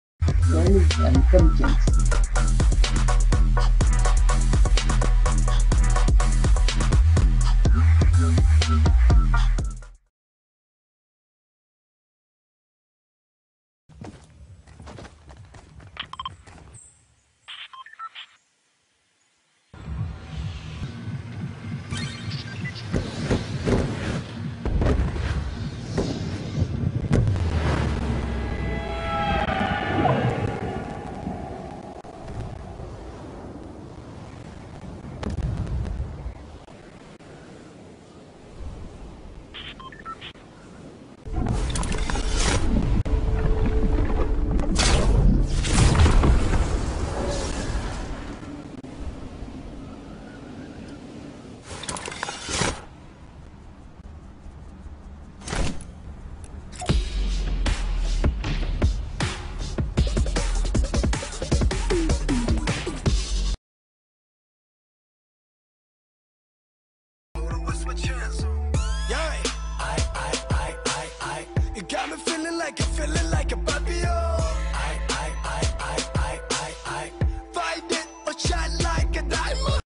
SAISON X Semaine 3 : Utiliser une Faille
Salut, c'est moi Skulls&Pumpkins

Dans cette vidéo je vais vous montrer comment faire le défi " Utiliser une Faille "

Bonne visualisation :D